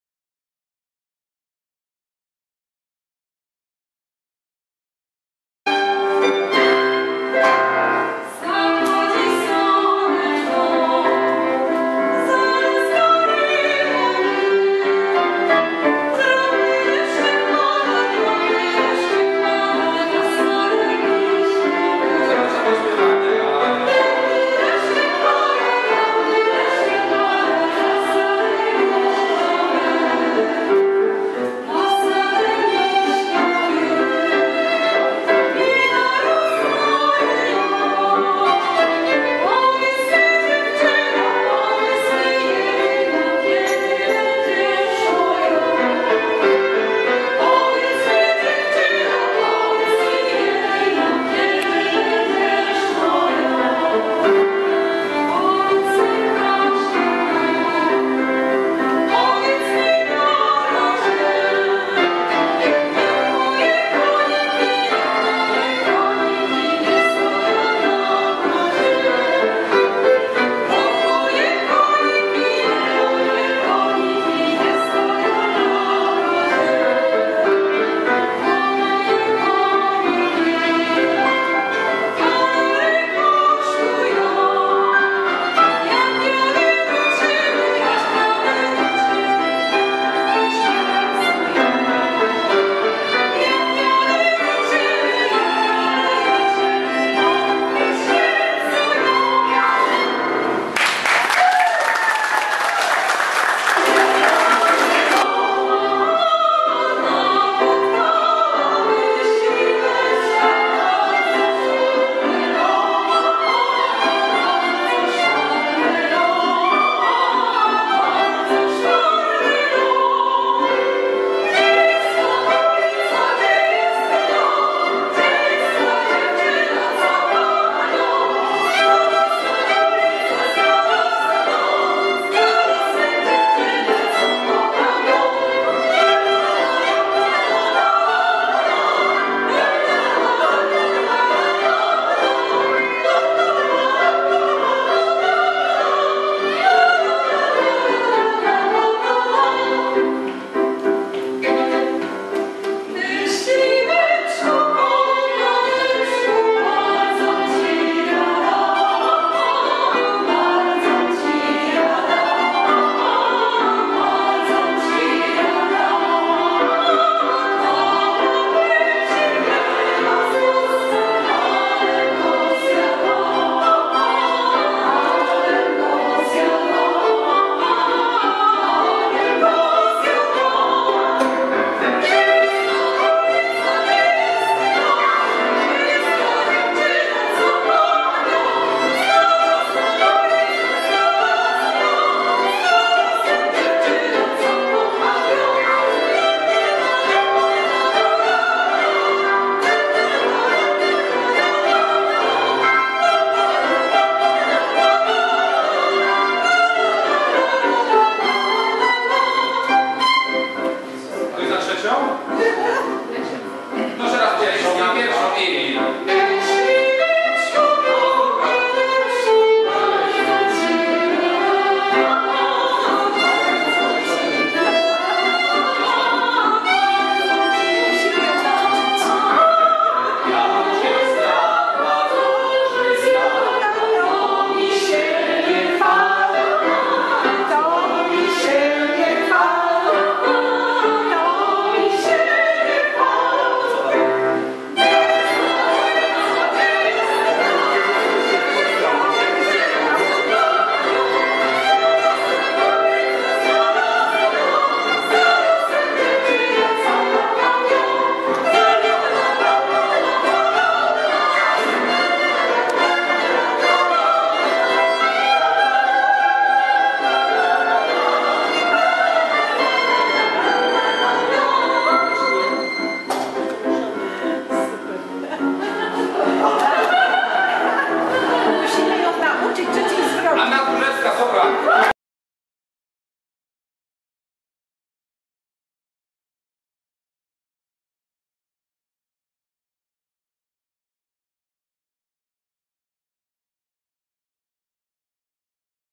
Anna Tchórzewska - 8.06.2015 Maszkiety
''Folk z naszych serc - koncert najpiękniejszych piosenek ludowych'' - "Maszkiety".

"Maszkiety" gościły dziś Annę Tchórzewską knurowiankę która występuje w Zespole Pieśni i Tańca "Śląsk" z Koszęcina. Anna Tchórzewska jest sopranistką oraz aktorką scen muzycznych, posiada dyplom studium Wokalno-Baletowego w Gliwicach z roku 2004.
W 2009 roku zrobiła licencjat na Akademii Muzycznej w Katowicach, specjalność wokalno-aktorska, a swoją przygodę ze śpiewem zaczynała w zespole folklorystycznym "Jedynka" z Knurowa by dalej kontynuować swoją muzyczną pasję w Chórze Kameralnym "Slavica Musa".
Anna Tchórzewska występowała również w Zespole Ludowym Pieśni i Tańca "Mazowsze" gdzie miała okazję szlifować swój warsztat muzyczny.
Dziś odbył się niepowtarzalny koncert pieśni ludowych, włączyła w ten klimat gości kawiarni " Maszkiety" którzy już nie mogą się doczekać kolejnego występu, którego będą mogli wysłuchać przy filiżance niepowtarzalnej kawy jaką serwują "Maszkiety".
Annie Tchórzewskiej akompaniowali dziś: Grzegorz Wojtasik - skrzypce oraz Robert Szewczuk - fortepian.